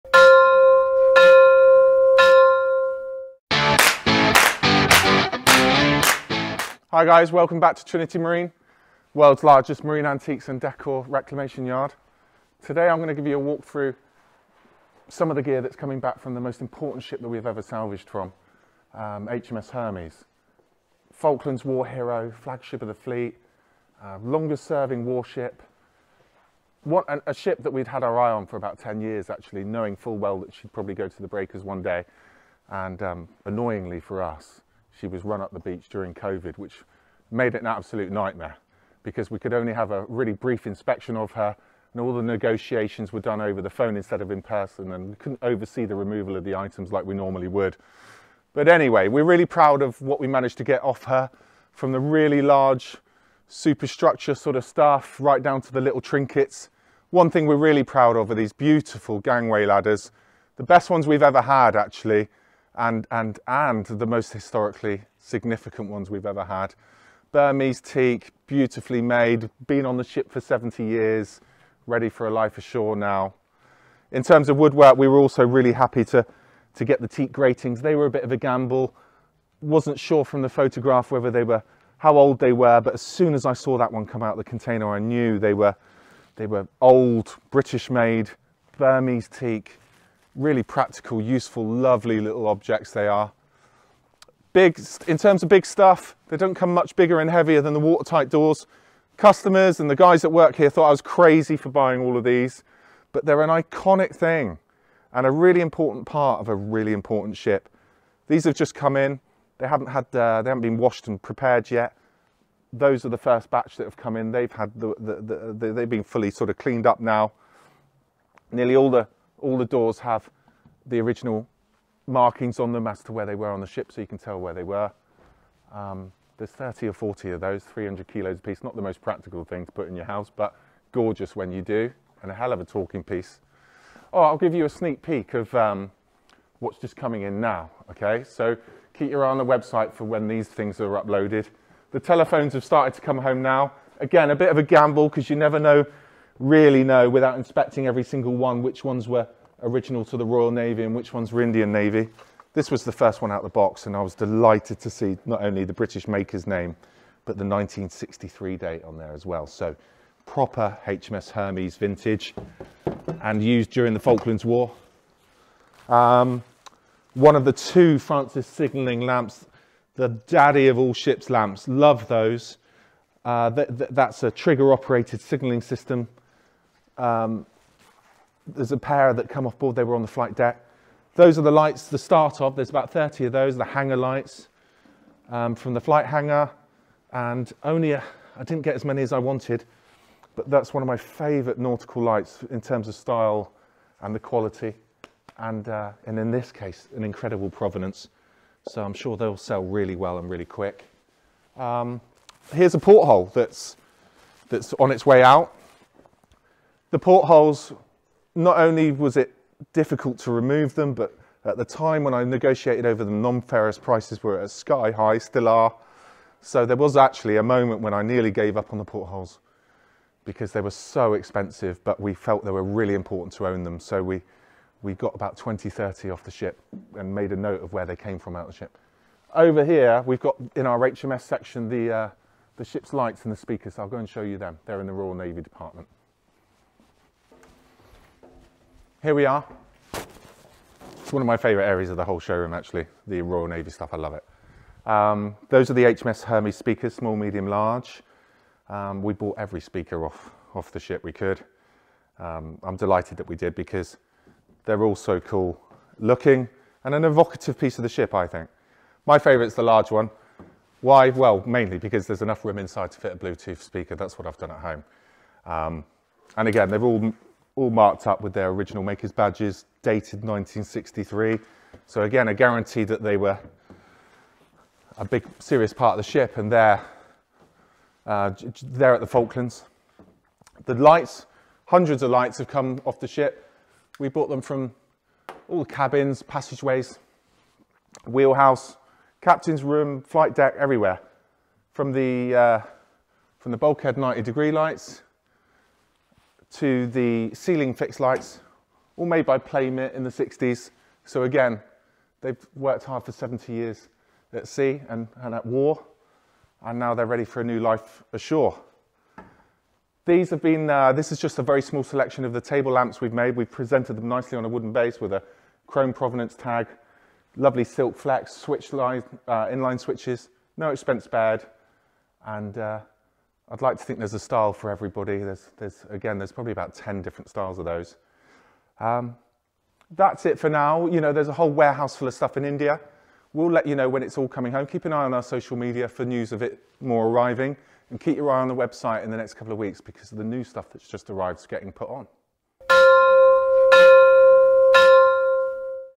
It's Maritime! ep.3 - The HMS Hermes Job
Mark takes us through some of the great pieces we have brought home from HMS Hermes and talks about the challenges and unusual circumstances of the reclamation of the last ever ship with quality salvageable fixtures and fittings. You will also get an exclusive viewing of some of the new Hermes pieces that have just arrived.